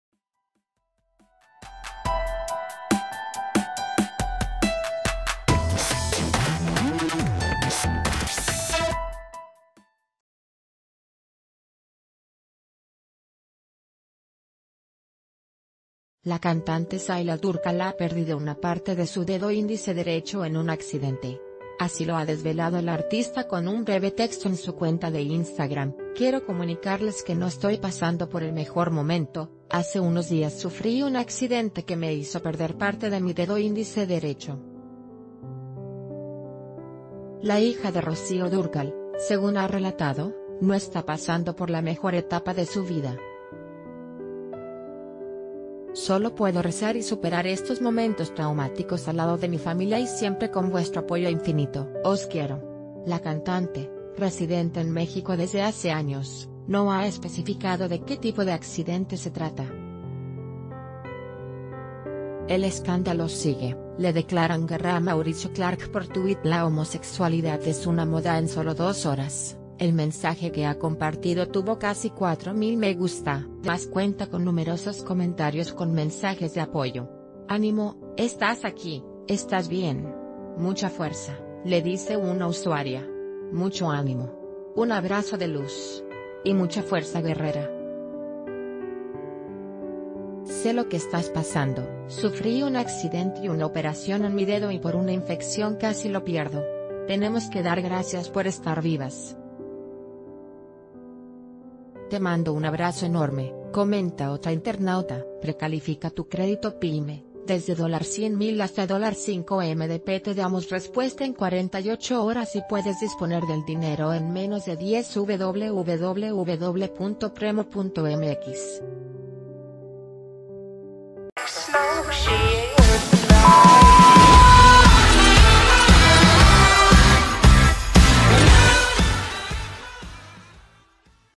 Vive momentos traumáticos Shaila Dúrcal tras accidente; perdió un dedo
La cantante Shaila Dúrcal ha perdido una parte de su dedo índice derecho en un accidente. Así lo ha desvelado la artista con un breve texto en su cuenta de Instagram:  "Quiero comunicarles que no estoy pasando por el mejor momento, hace unos días sufrí un accidente que me hizo perder parte de mi ded...